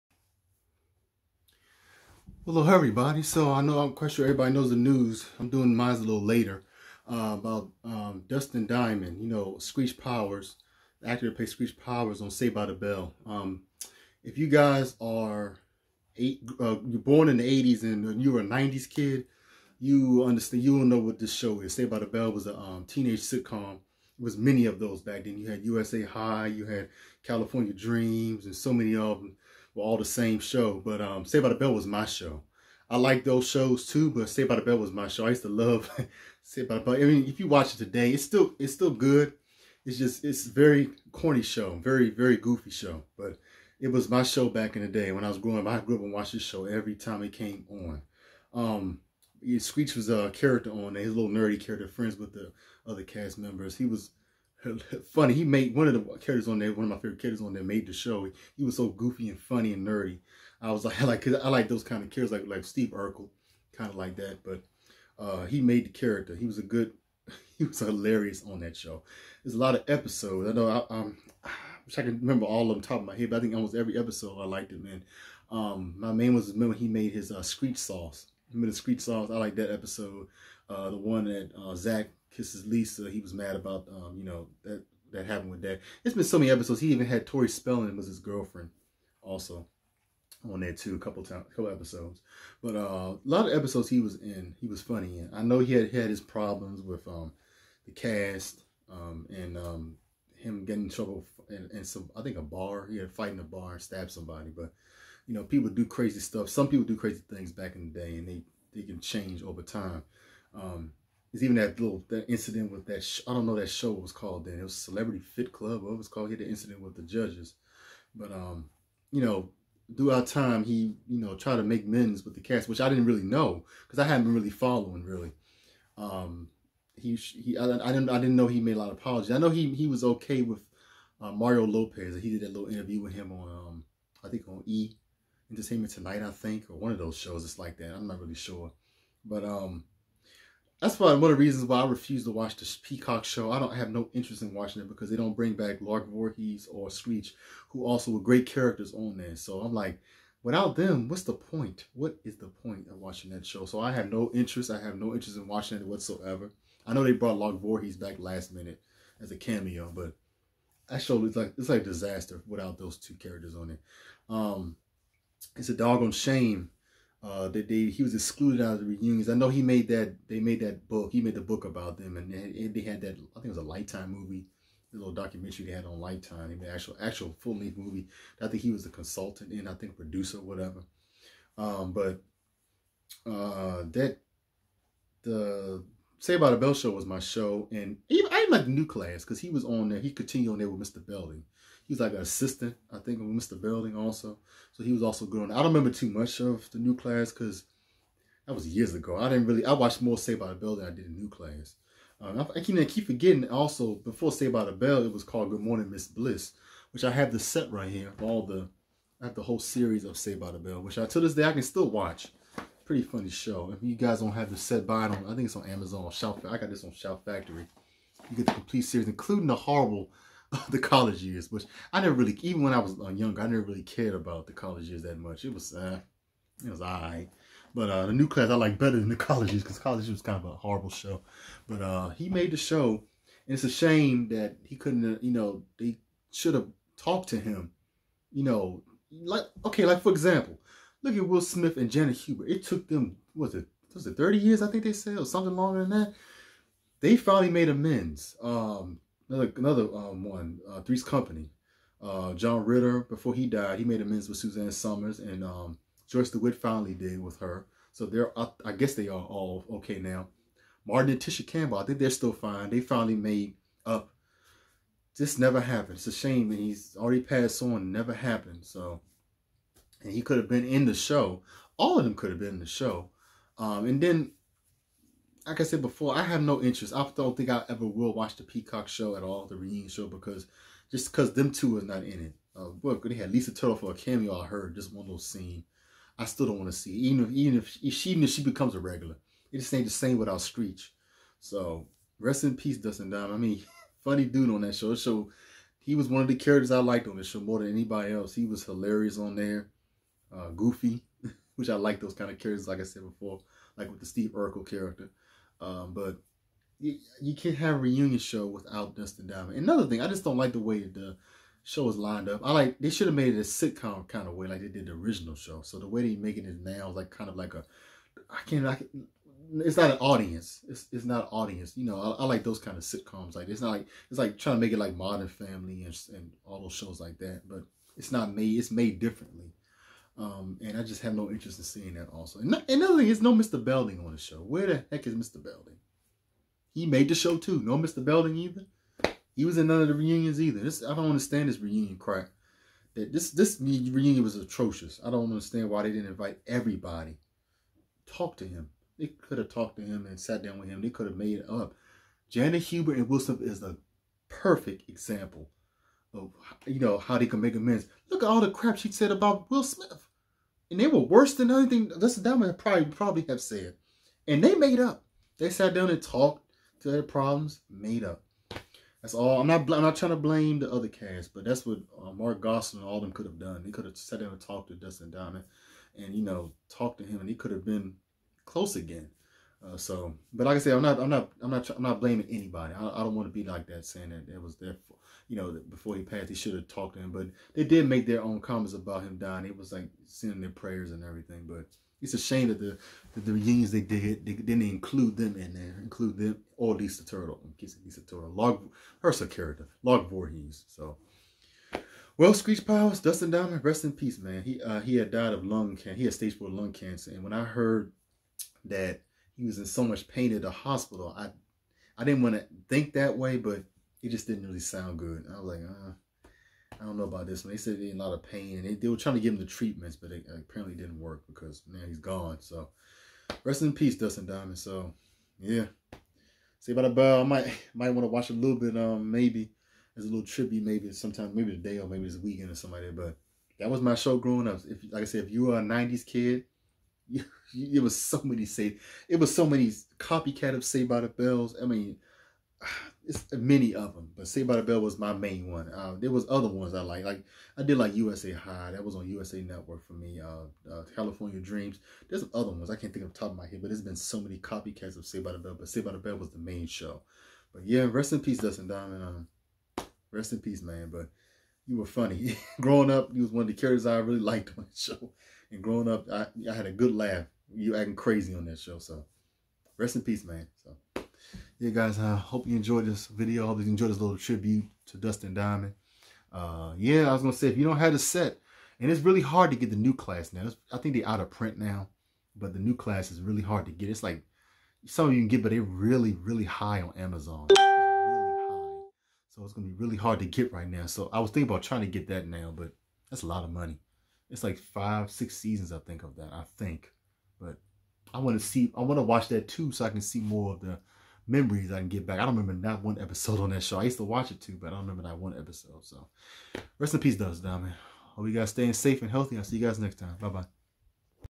DUSTIN DIAMOND DEAD AT 44. R.I.P. DUSTIN DIAMOND
Dustin Diamond aka Screech Powers, dies of lung cancer at 44. A tribute to Dustin Diamond.